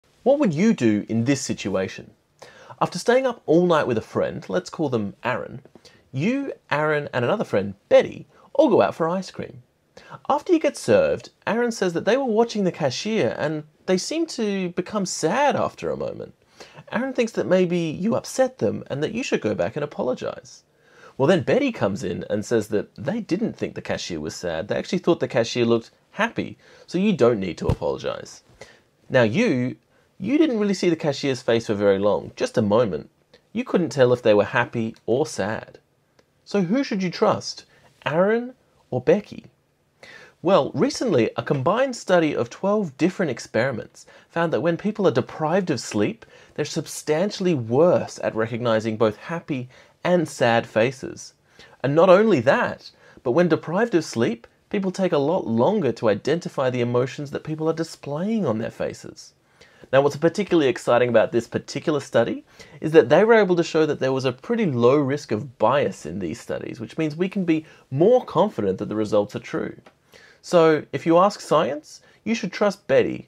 Do you TRUST your sleepy friends!?
When you're tired, you might find it harder to recognise happiness and sadness on people's faces!